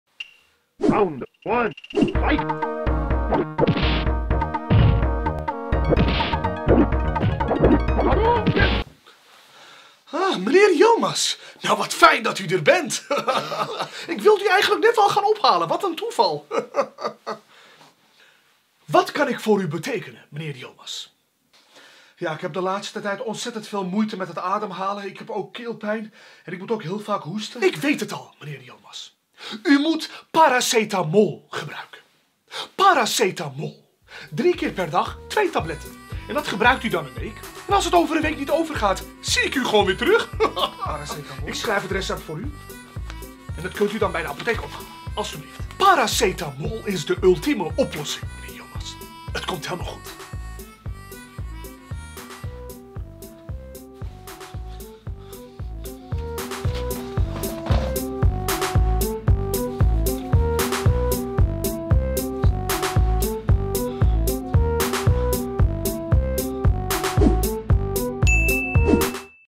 HUISARTS DRAMA (PART 2) | De Turan Show
Het ultieme recept van de huisarts, waar zelfs wereldberoemde koks van versteld staan.

volg @deturanshow op Instagram voor meer actuele filmpjes!